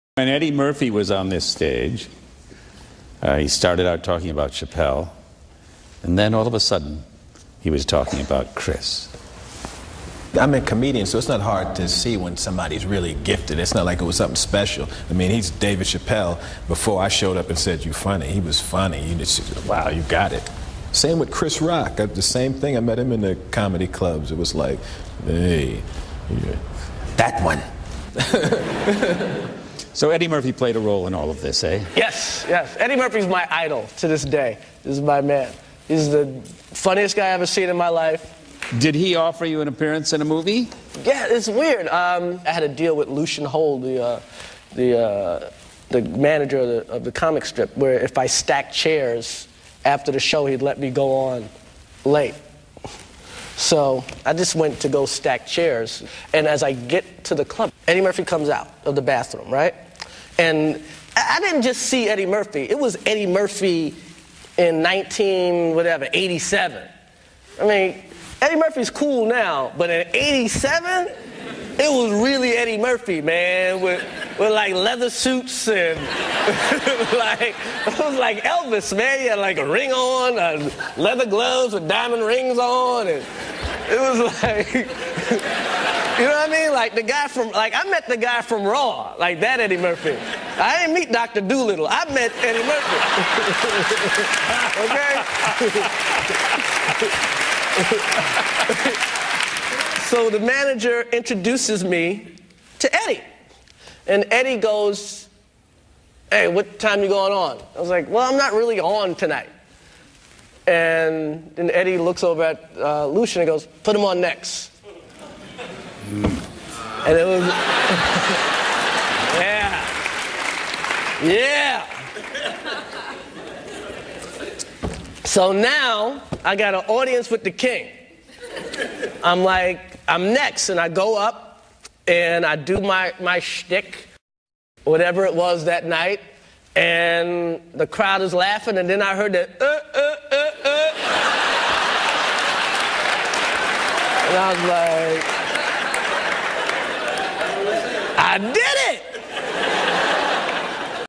Chris Rock Talking About Meeting Eddie Murphy In The 80s
80s, eddie murphy, chris rock, comedy, comedian, humour, jokes